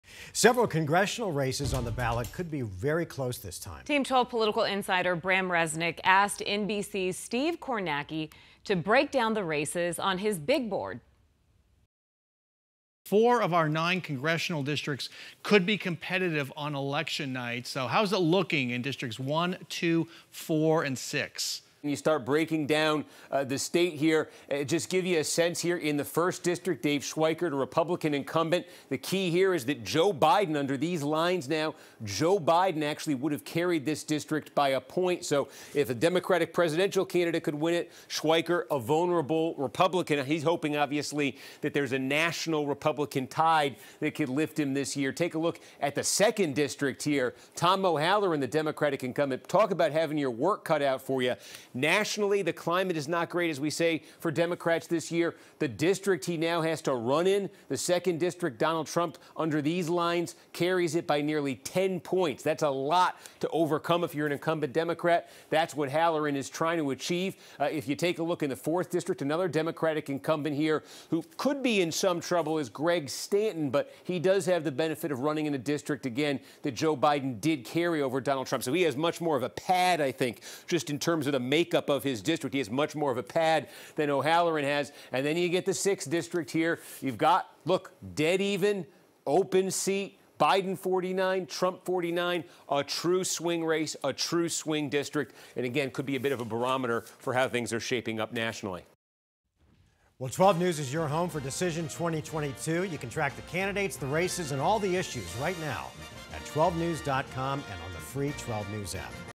Who will get elected in Arizona? Political journalist Steve Kornacki breaks it down
12News' Brahm Resnik asks political journalist Steve Kornacki to break down the upcoming race stats in key Arizona districts.